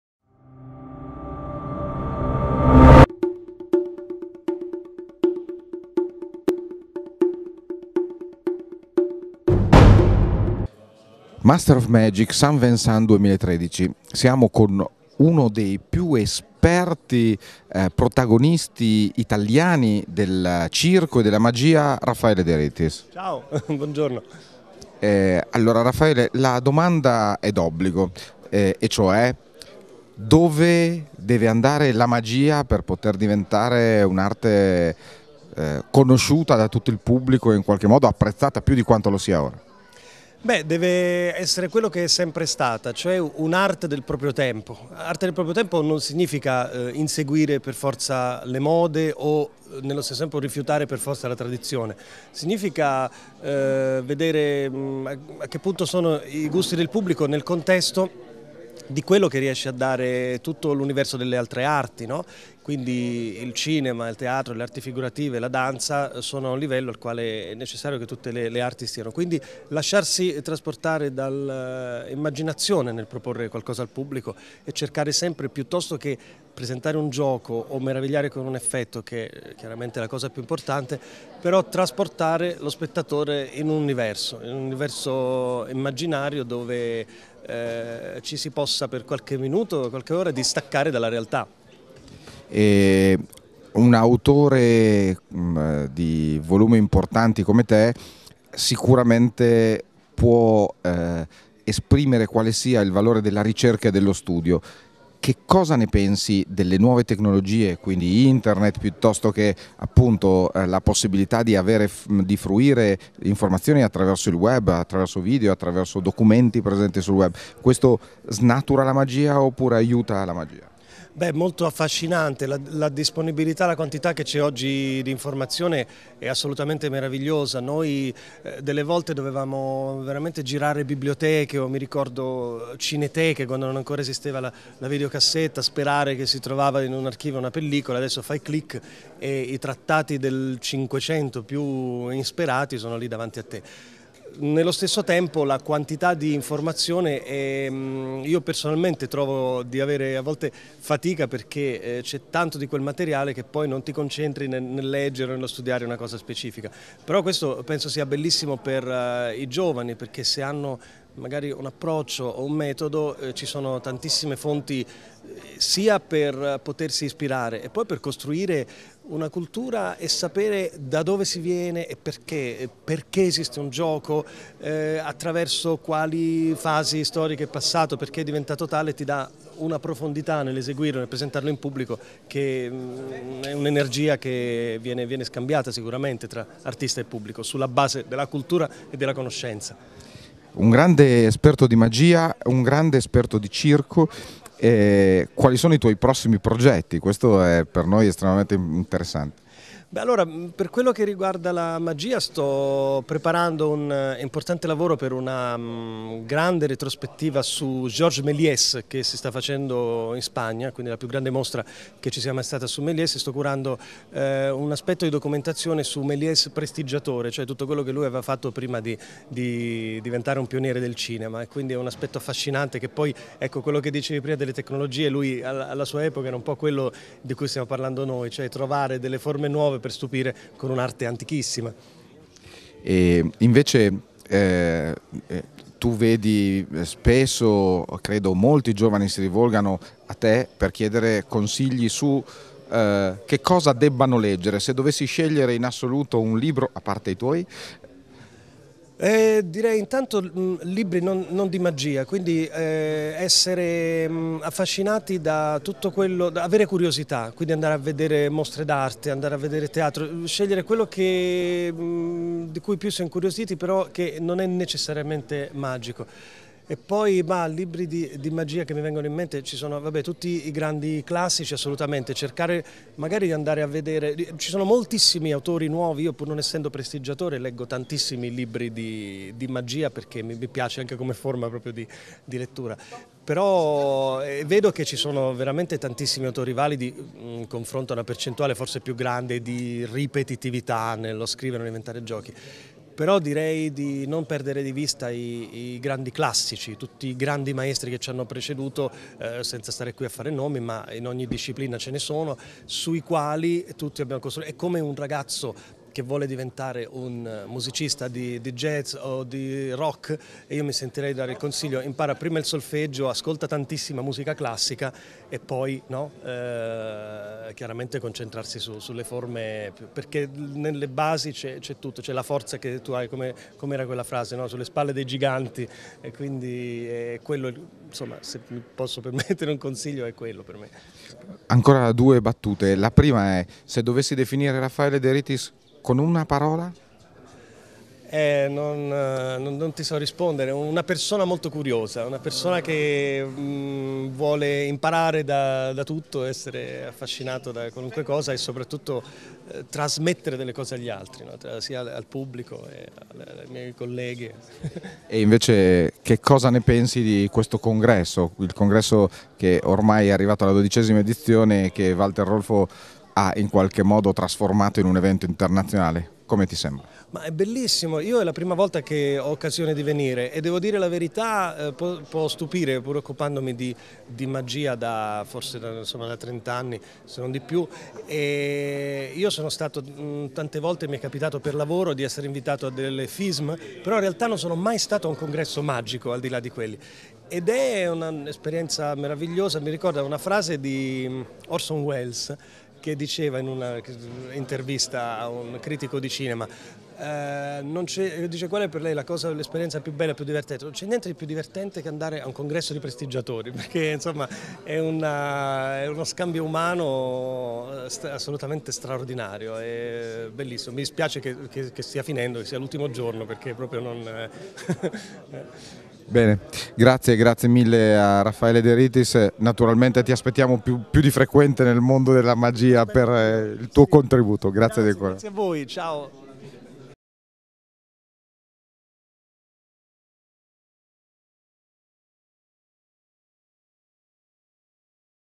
Raffaele De Ritis
Andrea Baioni intervista Raffaele De Ritis Al Congresso Magico di Saint VIncent 2013.